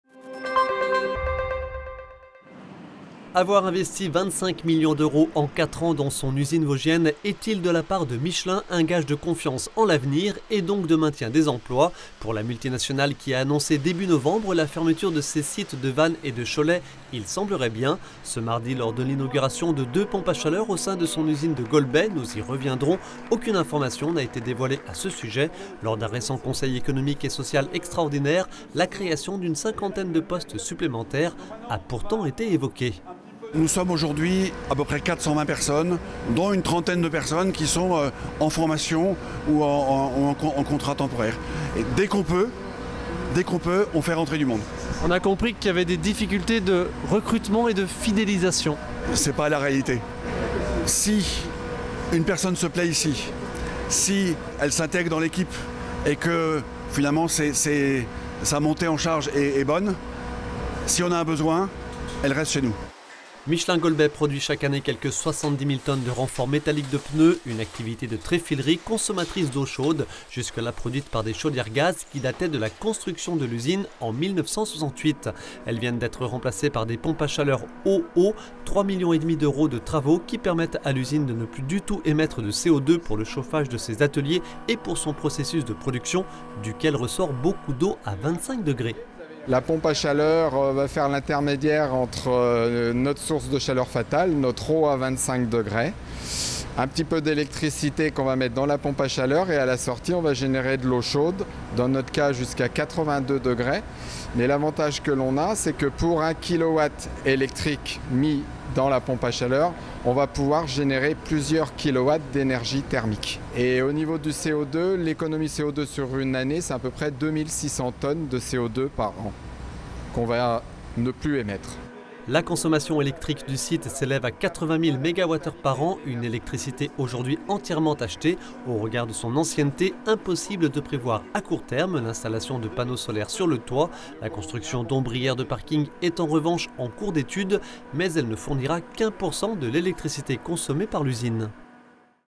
Rassurant ?
En remplaçant ses vieilles chaudières gaz par des pompes à chaleur eau-eau, l’usine Michelin de Golbey n’émet plus du tout de CO2 pour produire l’eau chaude qui chauffe ses ateliers et qui est utilisée dans son process industriel de tréfilage de renforts métalliques de pneus (70 000 tonnes par an). Un investissement de 3,5 millions d’euros (soit un global de 25 millions depuis 2020) dans un usine qui pourrait rapatrier une partie de l’activité de l’usine de Vannes dont la multinationale a récemment annoncé la fermeture. A la clé selon le syndicat SUD : une cinquantaine de nouveaux postes.